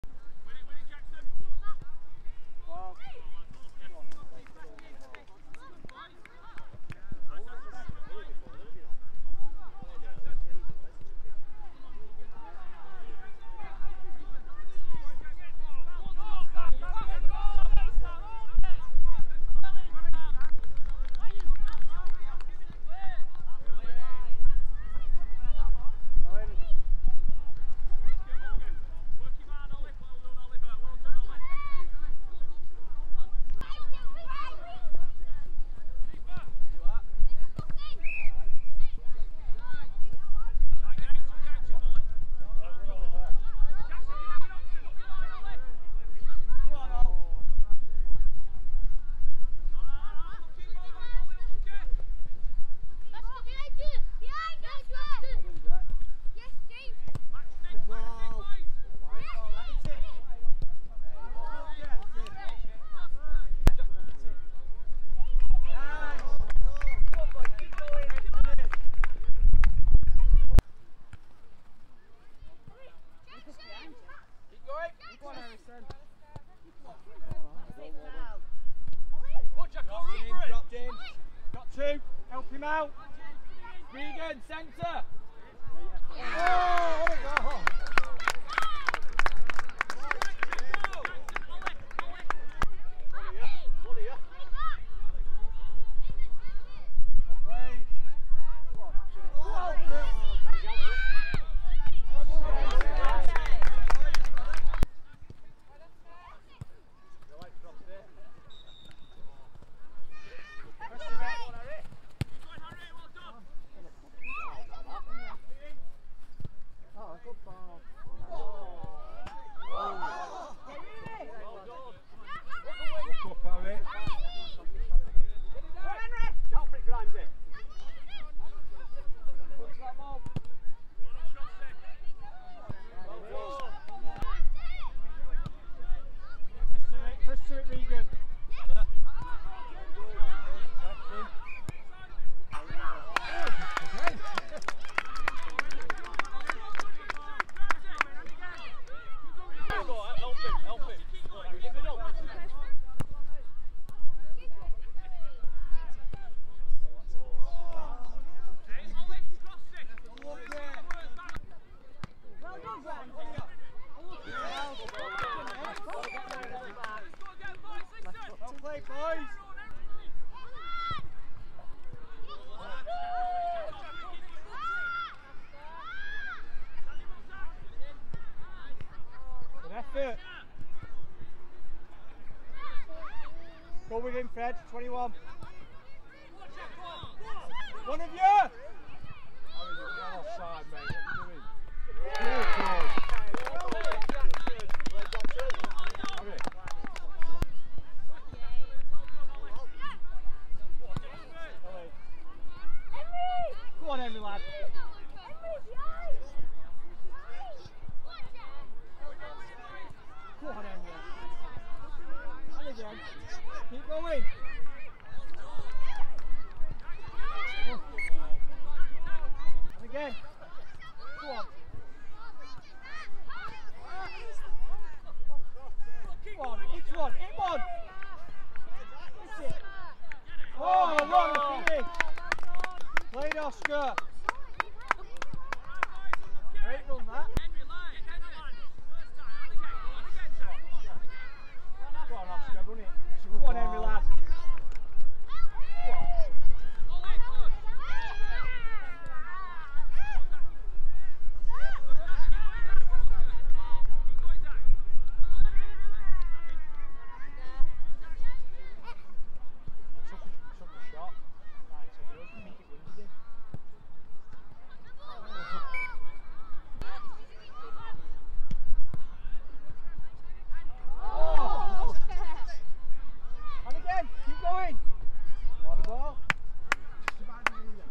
Roach Dynamos u10's v Junior Hoops, 03/10/2020 (3-3)
Roach Dynamos u10s play Junior Hoops (Haslingden) u10s on 3rd October 2020 in a North Bury & Bolton league fixture.
The final score was 3-3
Roach goal scorers:
Henry Riley (#3) - 2
Oscar Hopps (#5) - 1